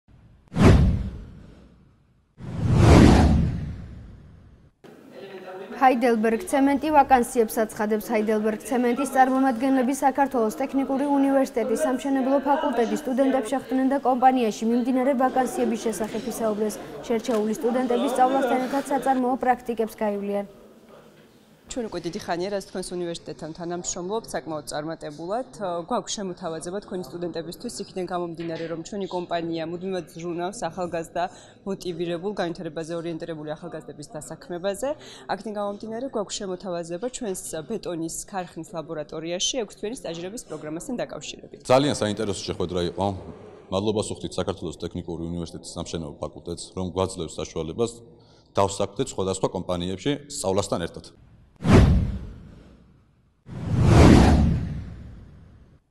ჰაიდელბერგცემენტი ვაკანსიებს აცხადებს
ჰაიდელბერგცემენტი ვაკანსიებს აცხადებს. ჰაიდელბერგცემენტის  წარმომადგენლები  საქართველოს  ტექნიკური უნივერსიტეტის  სამშნებლო ფაკულტეტის  სტუდენტებს  შეხვდნენ  და კომპანიაში  მიმდინარე  ვაკანსიების შსახებ ისაუბრეს . შერჩეული  სტუდენტები სწავლასთნ ერთად კომპანიაში საწარმოო პრაქტიკებს გაივლიან